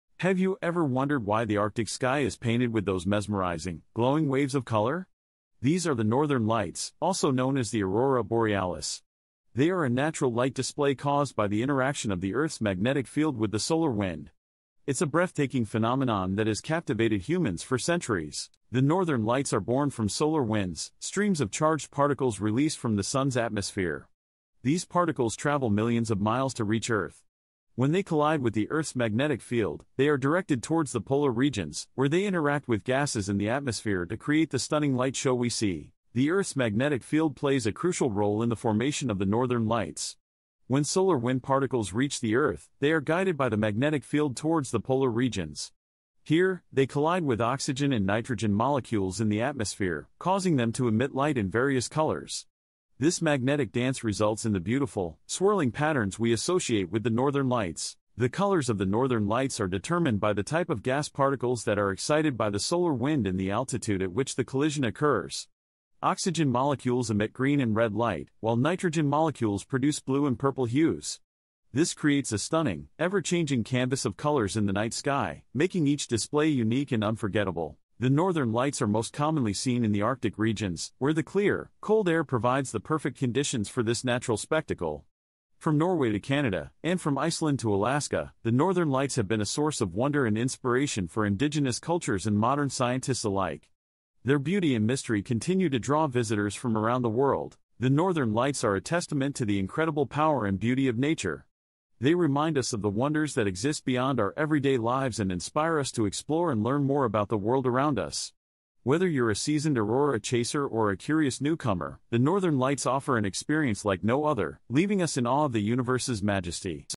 The Northern Lights 🌌 | The Science and Magic of the Aurora Borealis Explained
✨ High above the Arctic, the sky comes alive.
The Northern Lights, or Aurora Borealis, are one of Earth’s most spectacular natural wonders — a glowing dance of color across the night sky.
But what causes this dazzling light show? 🌍

In this video, we’ll explore the science, beauty, and mystery behind this phenomenon that has fascinated humans for centuries.

Here’s what you’ll discover:
☀️ Solar Winds — Streams of charged particles travel from the Sun to Earth, setting the stage for this cosmic performance.
🧭 Magnetic Dance — The Earth’s magnetic field directs these particles to the poles, creating the swirling arcs of color we see in the sky.
🎨 Colorful Canvas — Green, pink, purple, and red — each hue tells a story about the gases in our atmosphere and their altitude.
❄️ Arctic Beauty — From Norway and Iceland to Alaska and Canada, discover the best places to witness nature’s most magical light show.
💫 Eternal Wonder — The Aurora Borealis reminds us how small we are — and how magnificent our planet truly is.

📚 Chapters:
0:00 Aurora Alert — What Are the Northern Lights?
0:20 Solar Winds — The Sun’s Role
0:40 Magnetic Dance — How Earth Shapes the Lights
1:00 Colorful Canvas — What Do the Colors Mean?
1:20 Arctic Beauty — Where to See Them
1:40 Eternal Wonder — Nature’s Masterpiece

💬 “The sky is not just above us — sometimes, it’s alive.”